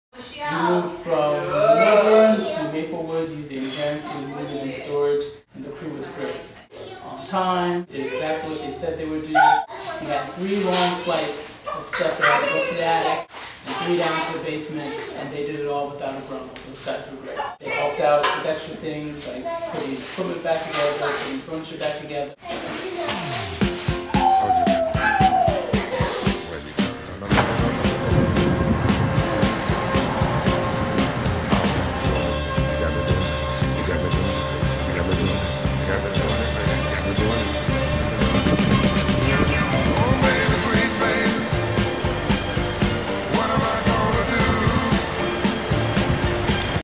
Millburn NJ Moving Team
Harrington Moves Millburn NJ Family to Maplewood NJ. A Difficult local move made easy by Harrington Millburn Movers. Three flights, disassemble and reassemble furniture.